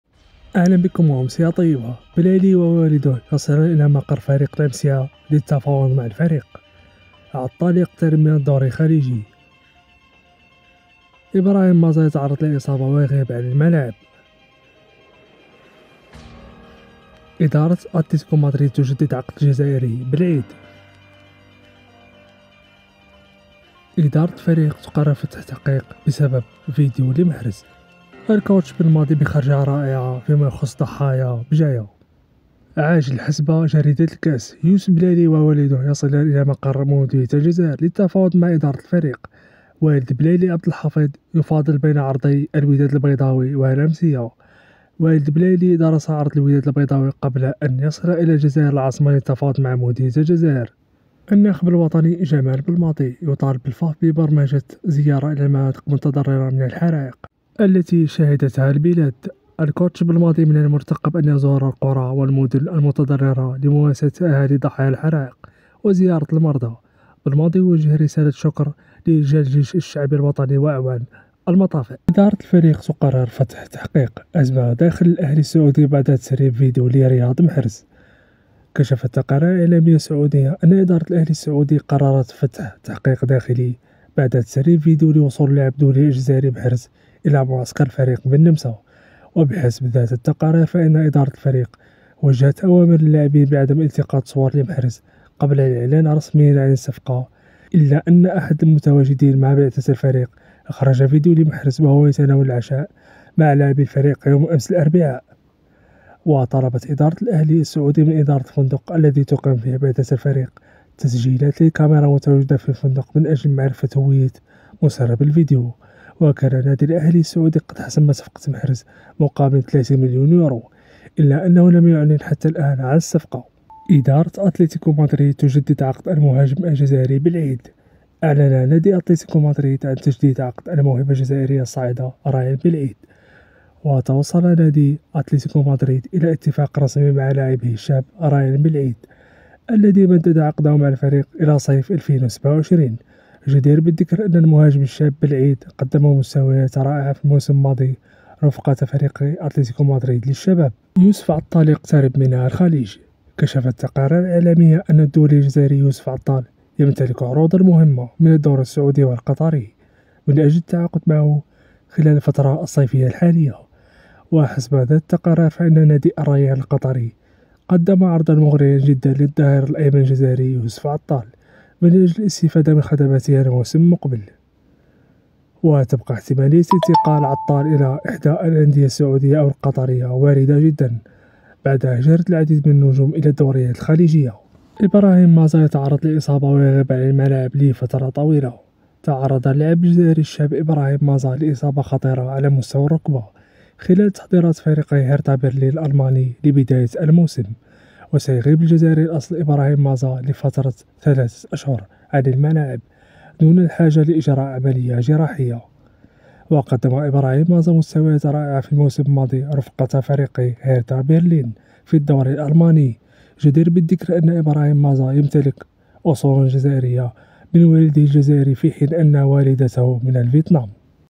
عاجل بلايلي يصل إلى مقر فريق MCA للتفاوض. عطال قريب من الريان بلماضي يقرر دعم المتضررين من حرائق
كأس بن سلمان للأندية العربية
الإتحاد الترجي
جمال بلماضي
حرائق
بجاية
انتهت: أهلي طرابلس الليبي 0-0 الهلال السعودي